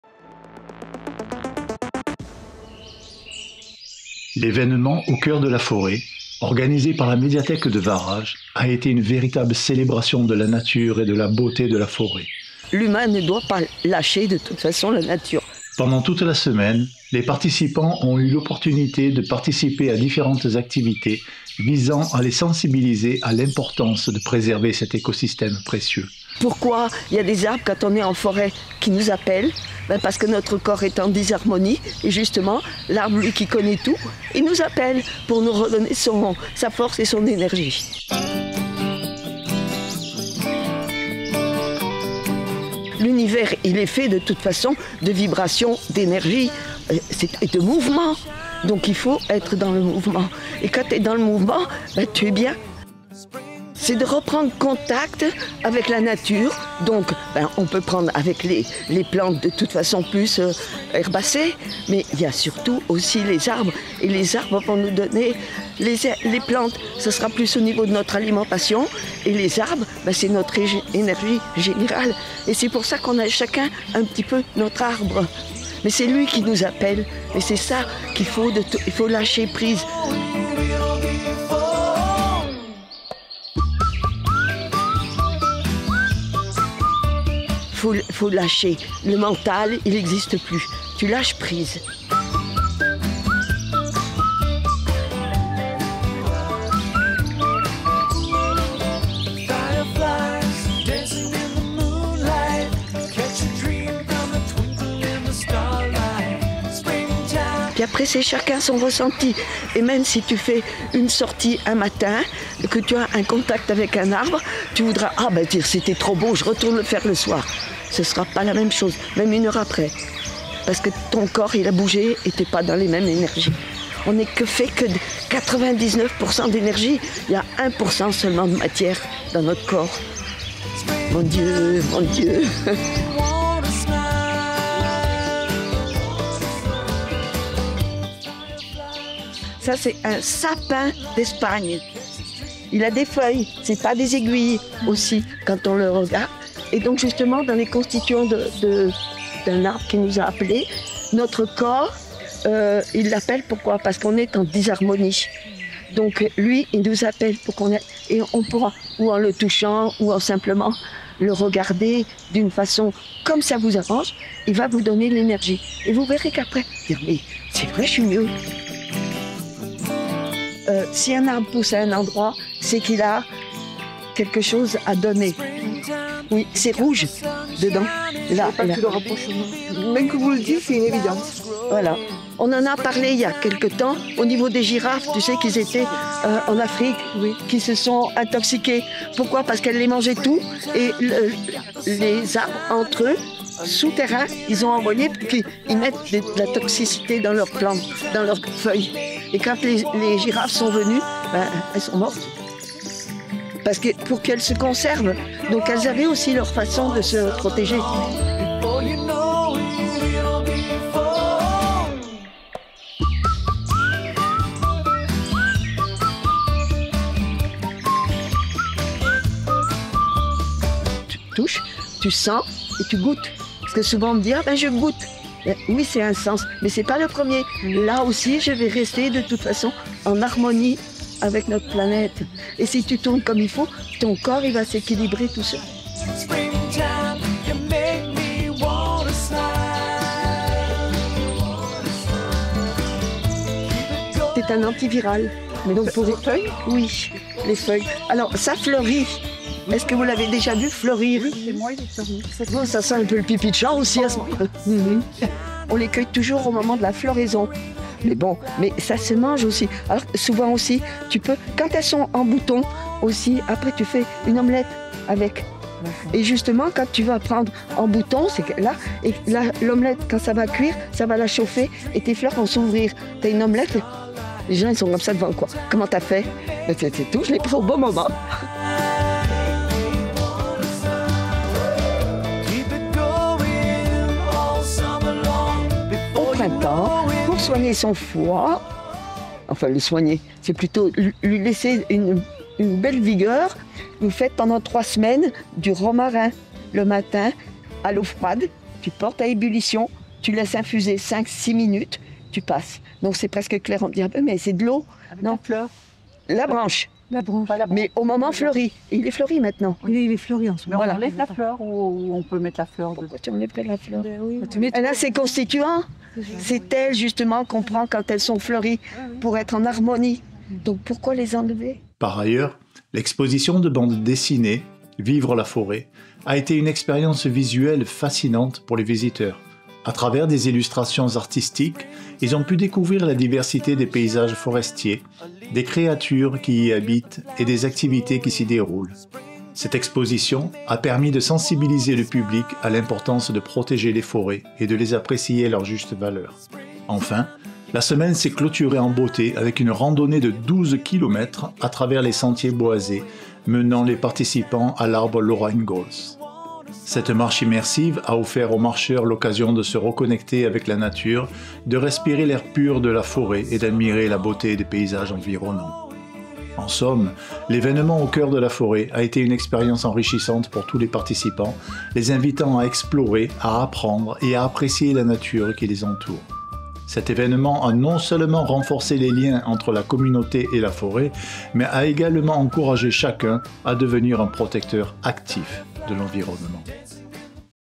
Au Coeur de la Forêt
Une semaine immersive sur le thème de la forêt organisée par la Médiathèque de Varages.